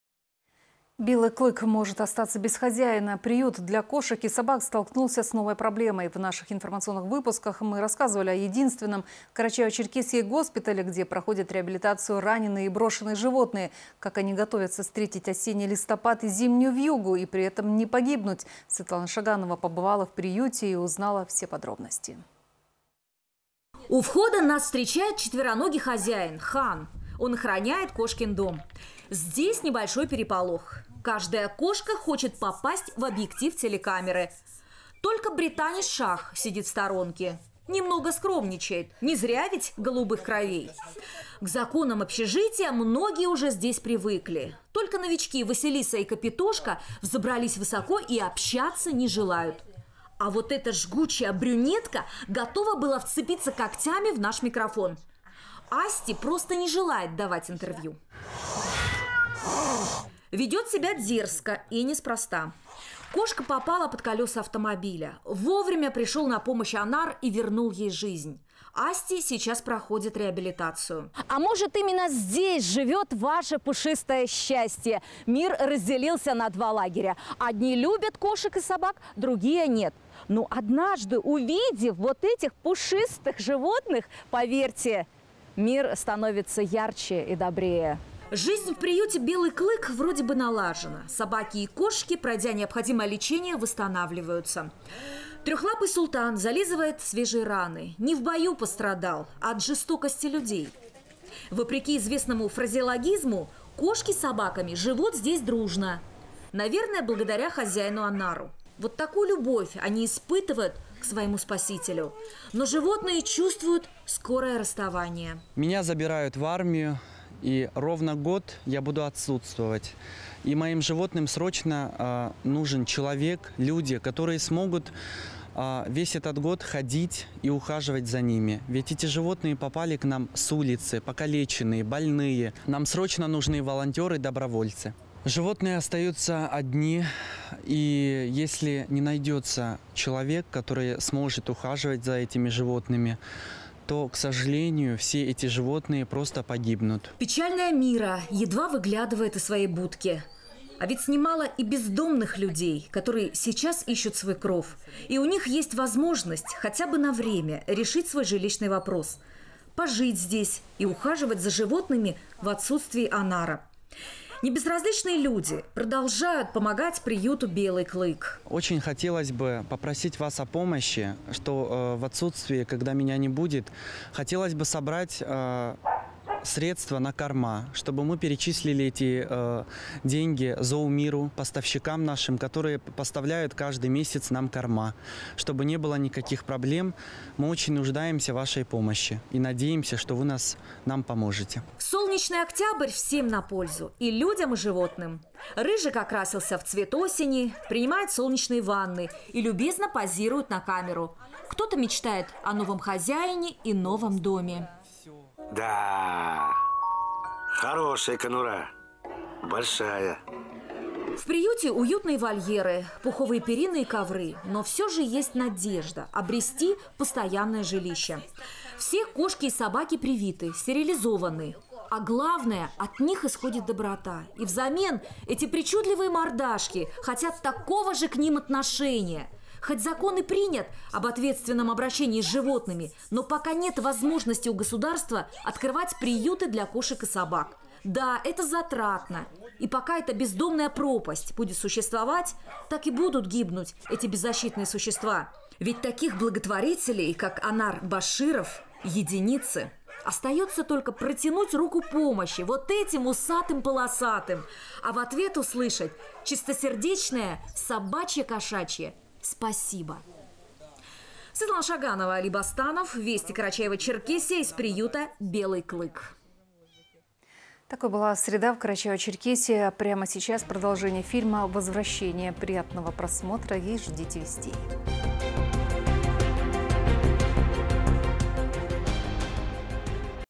Приют для животных "Белый клык" может остаться без хозяина!
"Белый клык" может остаться без хозяина! Приют для кошек и собак столкнулся с новой проблемой. В наших информационных выпусках мы рассказывали о единственном в Карачаево-Черкесии госпитале, где проходят реабилитацию раненые и брошенные животные